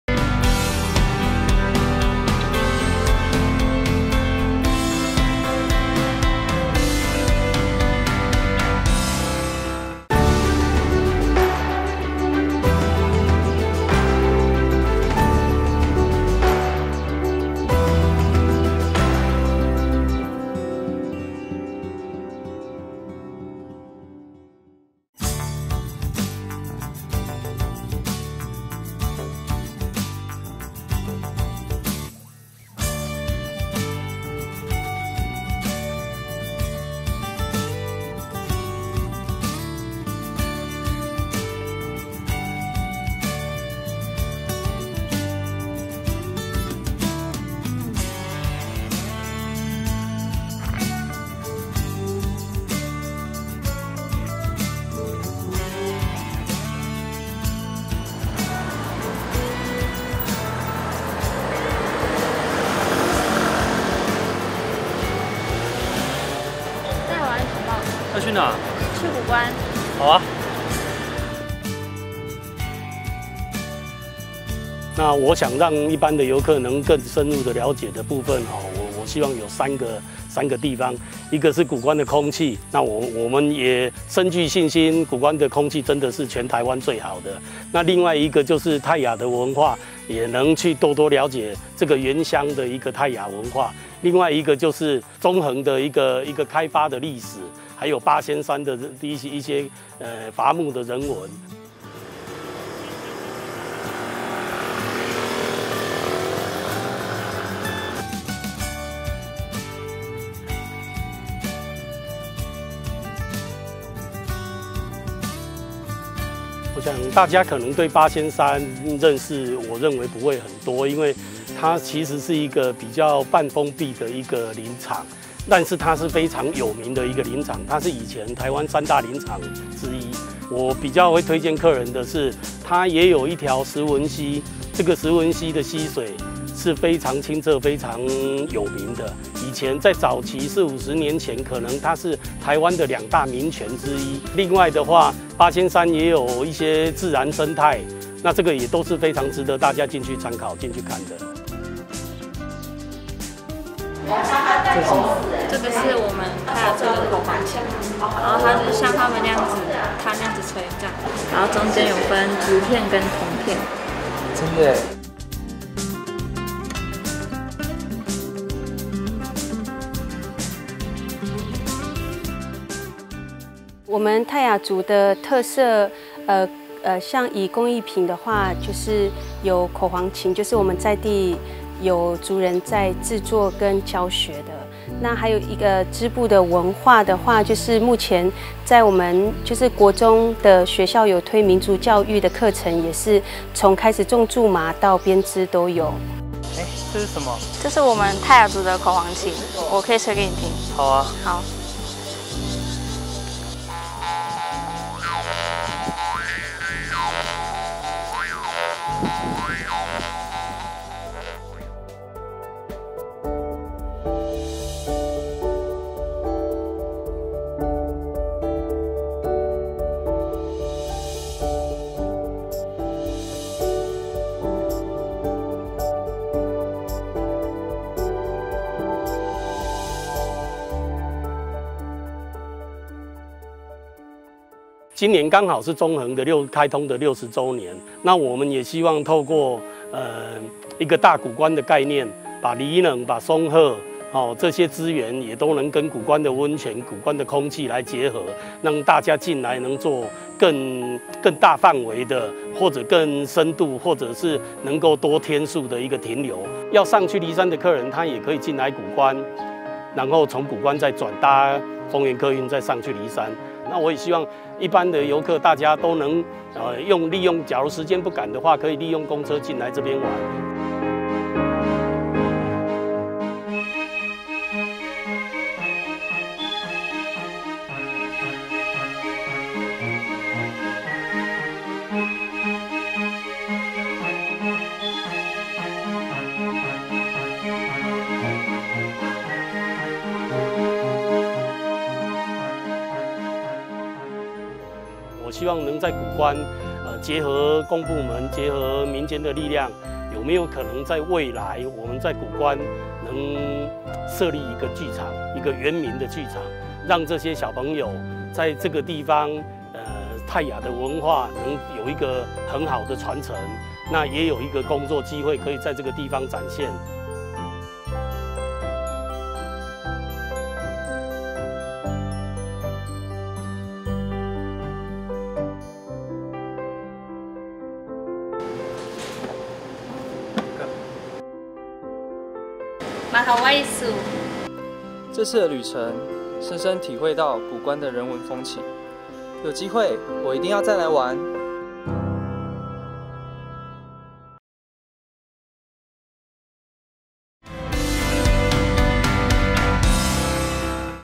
發現新台灣 台中市和平區谷關商圈形象宣導影片
【台中市和平區谷關社區發展協會】
遊子返鄉 攜伴回家
漢人融合原民文化
從中橫公路到谷關
空氣 人文 泰雅文化
互相連結在一起
享受谷關社區的美麗
未來串連周邊的社區
共同發展大谷關社區